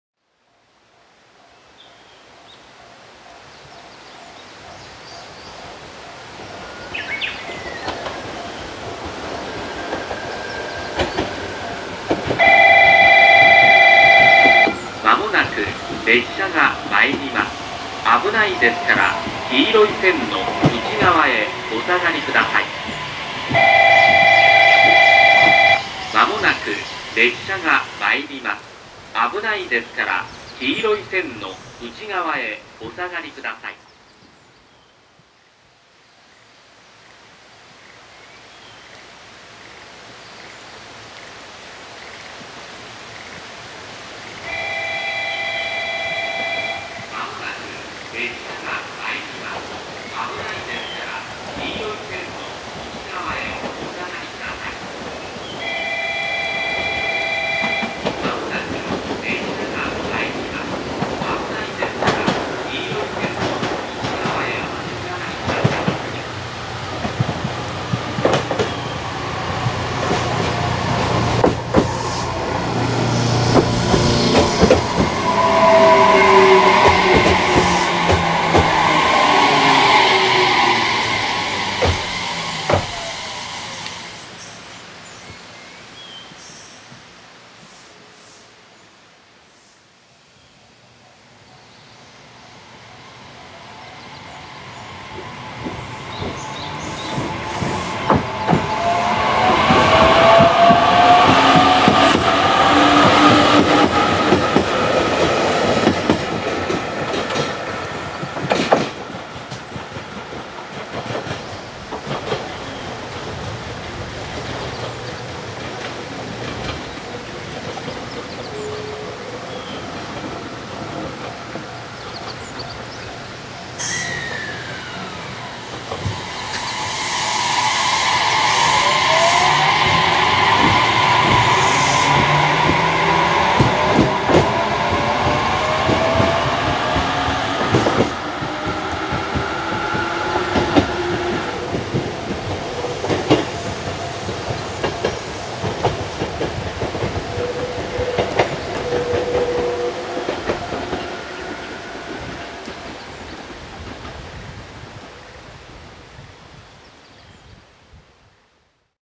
JR九州日豊本線 北永野田駅 817系交換 [2016.05.09]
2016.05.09 霧島市霧島にて撮影
日豊本線北永野田駅
16:27の交換
上り：817系/6946M
下り：817系/6955M